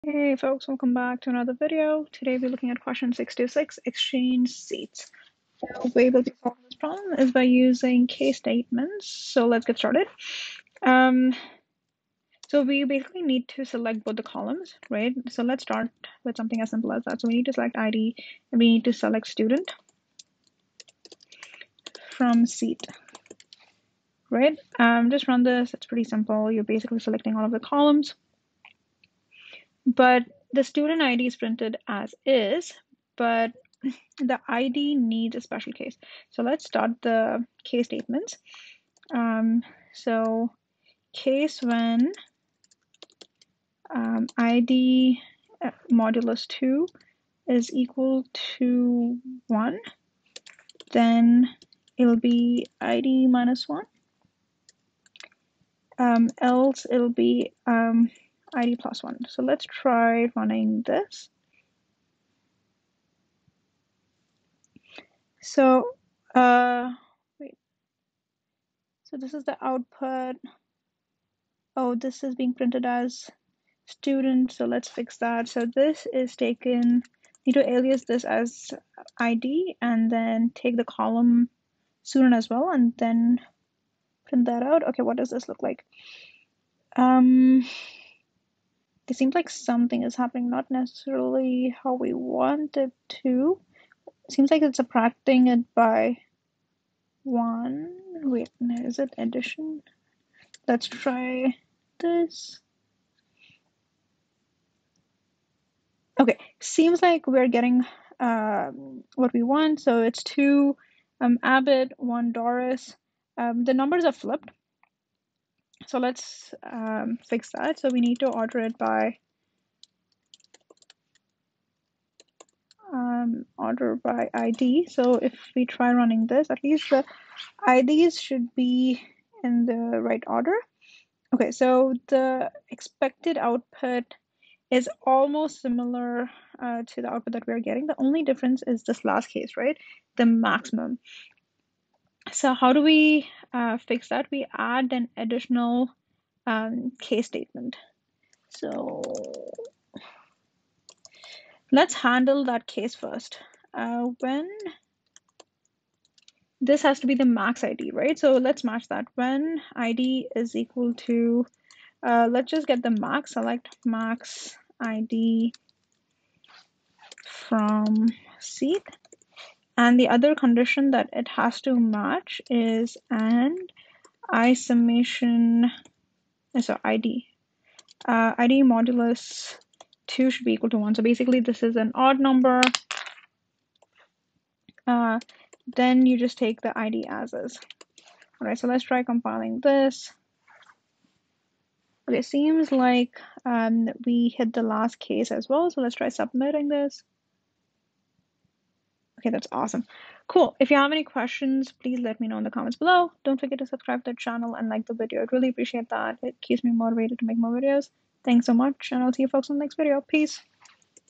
626. Exchange Seats (LeetCode, SQL)
Question 626 on LeetCode solved in MySQL